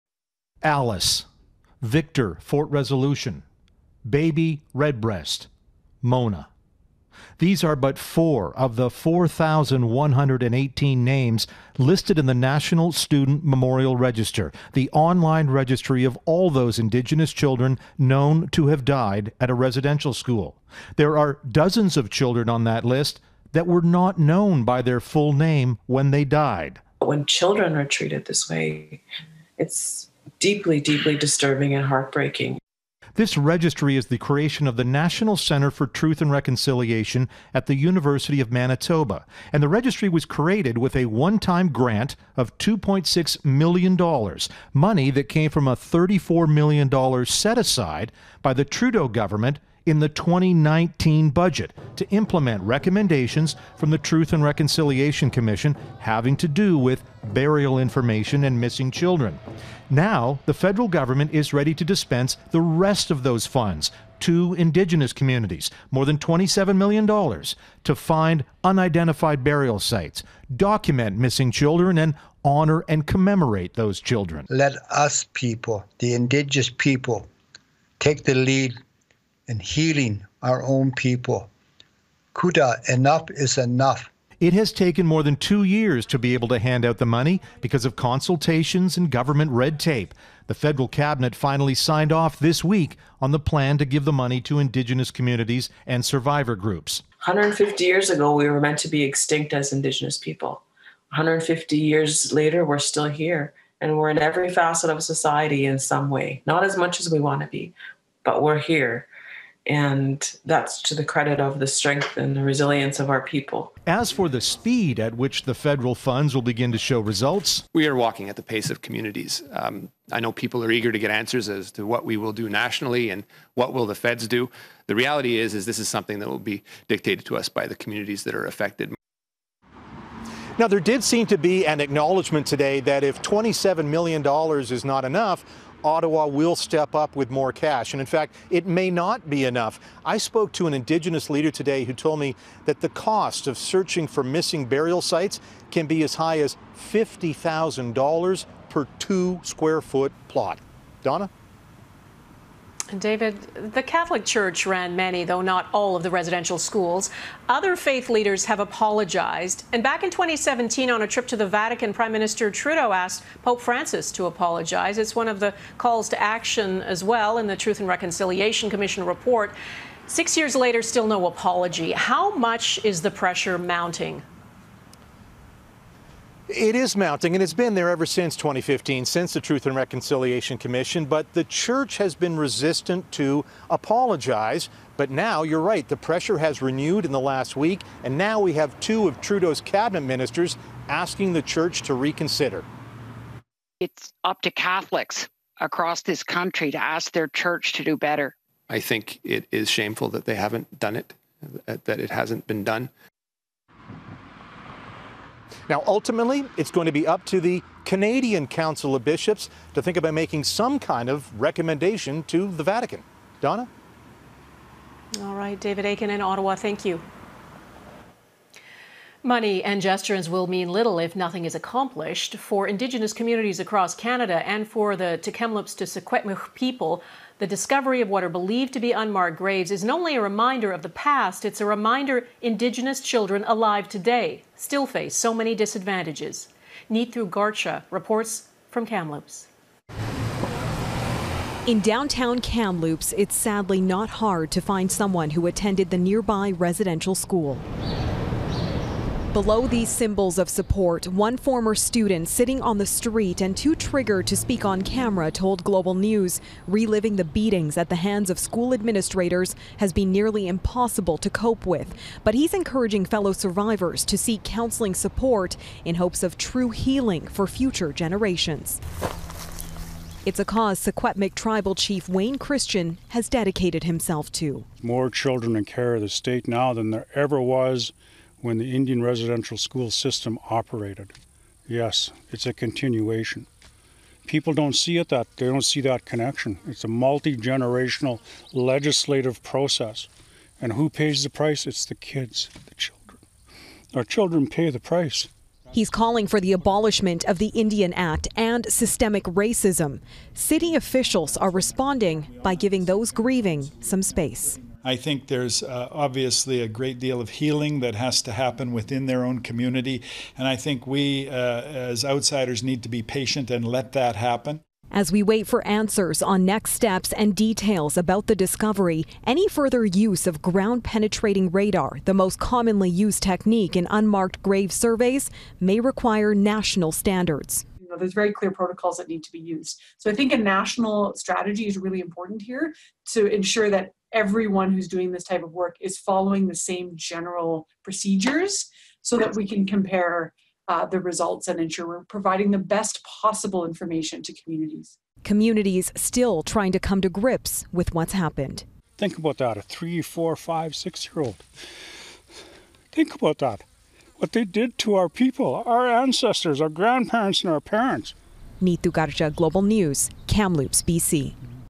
Indigenous communities to receive federal funding to search for other burial sites
The federal government is giving the remainder of the $34-million promised to Indigenous communities in the 2019 budget to implement recommendations from the Truth and Reconciliation Commission having to do with burial information and missing children.

Ottawa says the hand out of more than $27-million was delayed by consultations and government red tape. As David Akin reports, the feds appear to be ready to supply more money if these funds aren't enough.

And even though residential schools have been abolished, Indigenous children are over represented within Canada's foster care system.

As Neetu Garcha explains, that's one of the reasons Tk'emlúps te Secwépemc Tribal Chief Wayne Christian is calling for the abolishment of the Indian Act.